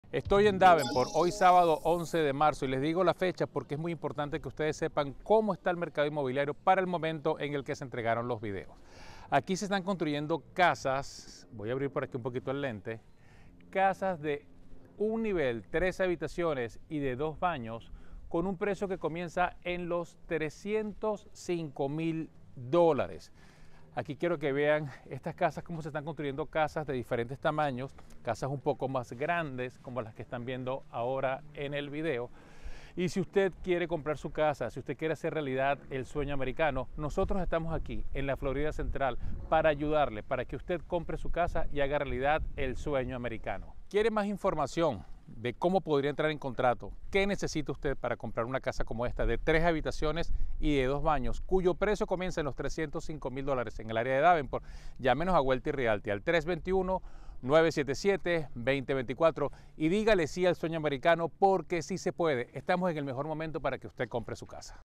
Casas desde $305,000 en Davenport, Florida. APROVECHA Reducción de Seguro Hipotecario (MIP)
ATENCION!!! Casas en CONSTRUCCION en Davenport, Florida!!!

Cuentan con 3 habitaciones y 2 baños, precio comienza $305,000 y hay ayuda para gastos de $10,000 e interés PREFERENCIAL.

Llámanos a Wealthy Realty al: (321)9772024 para más información.